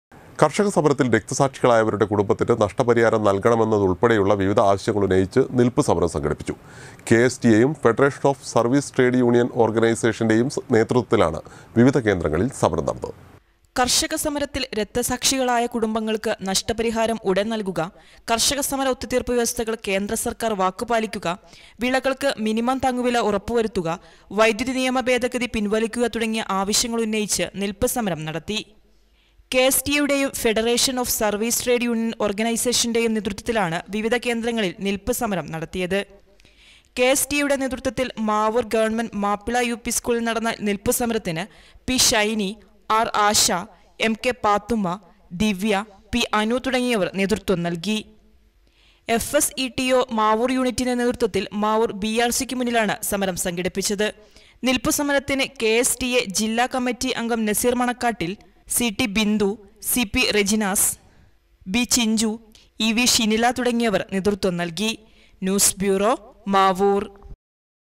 വിവിധ ആവശ്യങ്ങൾ ഉന്നയിച്ച് നിൽപ്പ് സമരം നടത്തി
കർഷക സമരത്തിൽ രക്തസാക്ഷികളായവരുടെ കുടുംബത്തിന് നഷ്ടപരിഹാരം നൽകണമെന്നതുൾപ്പെടെ വിവിധ ആവശ്യങ്ങൾ ഉന്നയിച്ച് നിൽപ്പ് സമരം നടത്തി. കെ.എസ്.ടി.എ, ഫെഡറേഷൻ ഓഫ് സർവീസ് ട്രേഡ് യൂനിയൻ ഓർഗനൈസേഷൻ്റെയും നേതൃത്വത്തിലാണ് വിവിധ കേന്ദ്രങ്ങളിൽ സമരം നടന്നത്